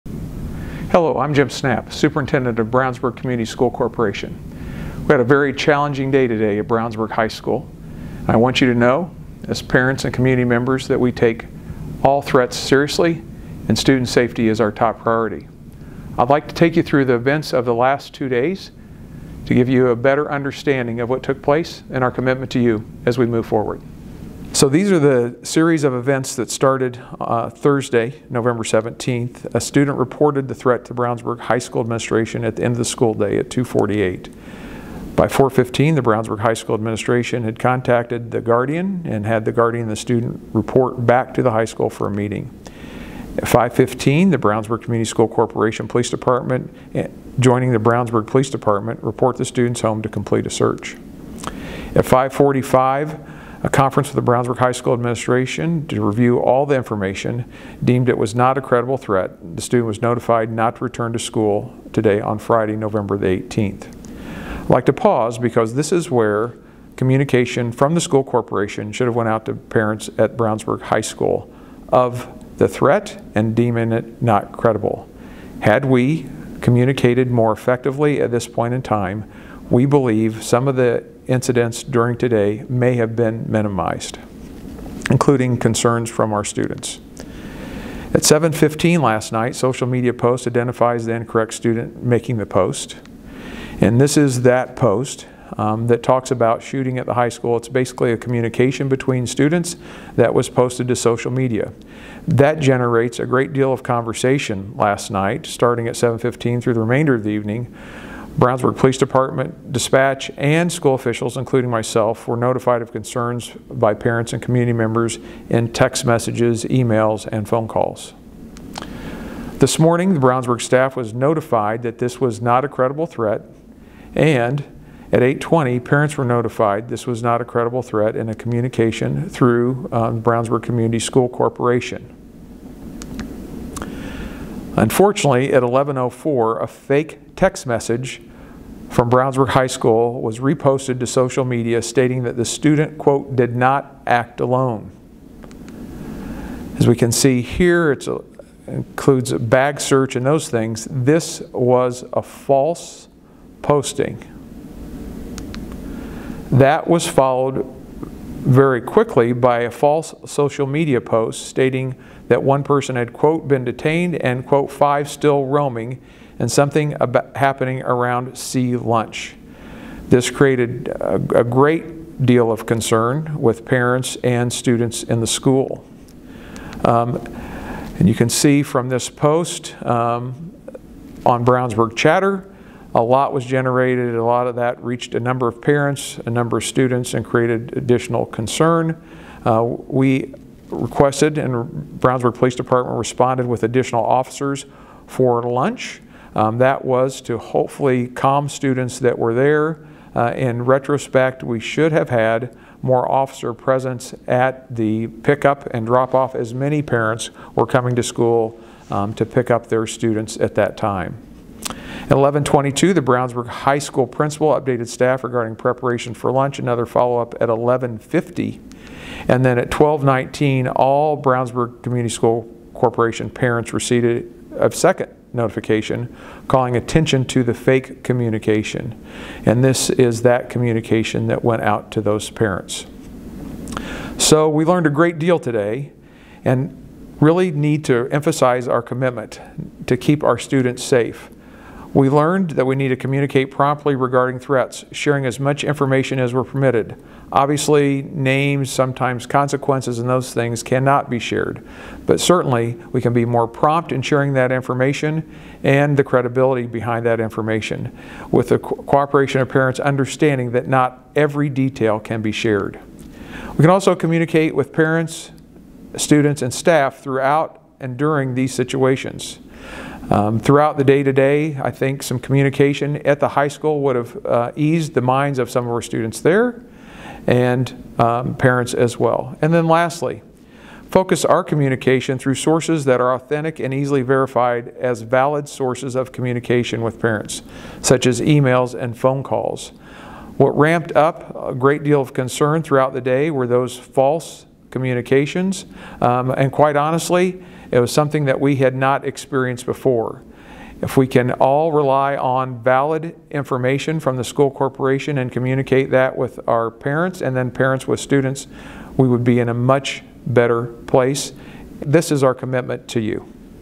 Information from BCSC - November 18, 2022
Superintendent Jim Snapp shares information with families and the community about a non-credible threat to BHS